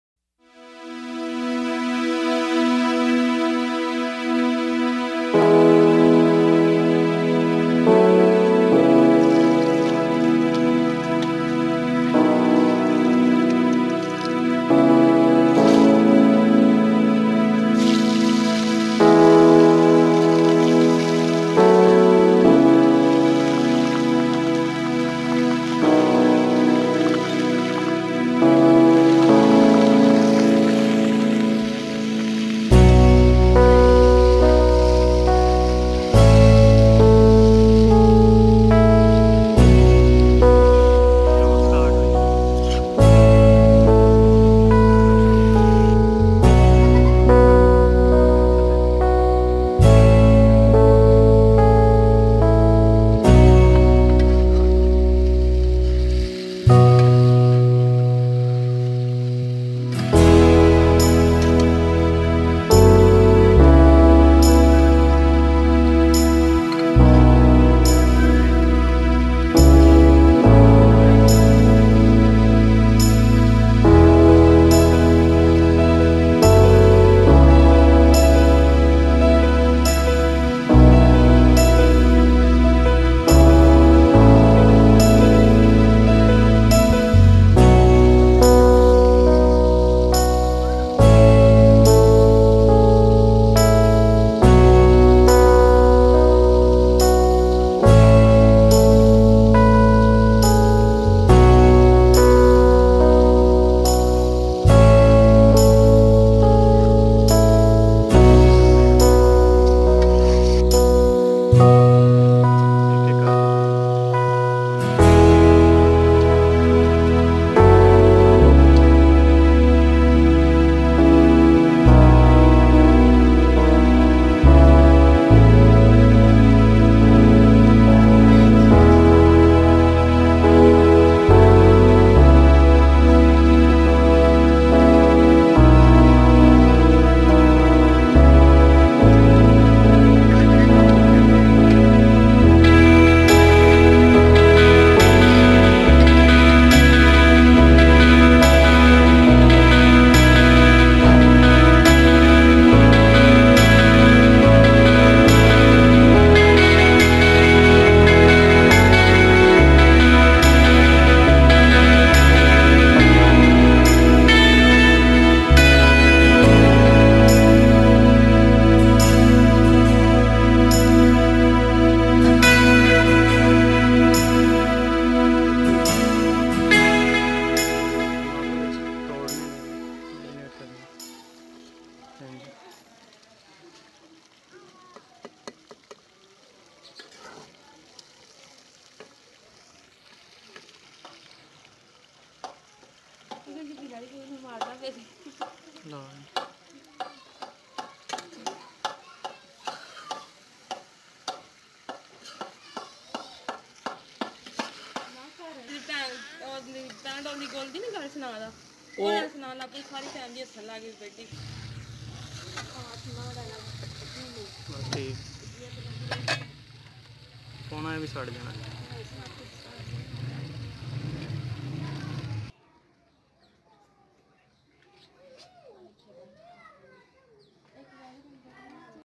Dawat wali chicken Kadhai village style recipe by Naeem Baloch food secret
Aaj ki is video Mein Humne banai Hai

Chicken kadahi ye ap dawat Dawat Mein

 banae Aur mehmanon Ko khilan Agar aapko video acchi Lage to like Kijiye share Kijiye

 Kijiye Hamare channel Naeem Baloch food secret ko